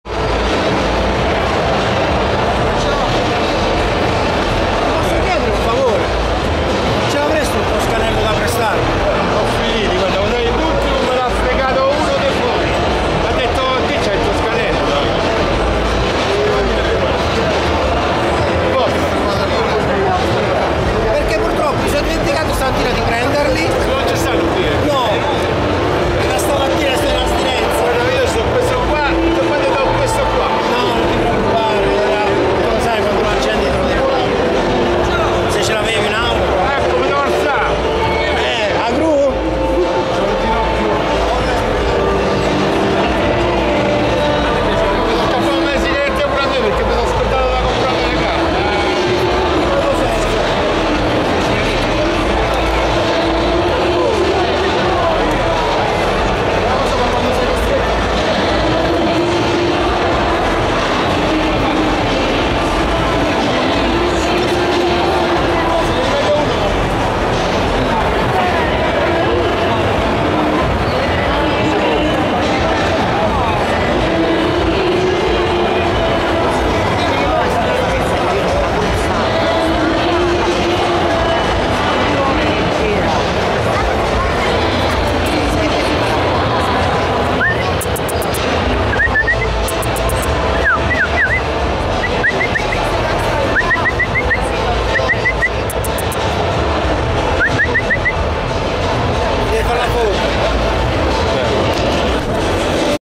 Custom Motorcycle & Boss Dog - Bike Tuning Show
Bike tuning show: a "boss" dog near its amazing custom motorcycle.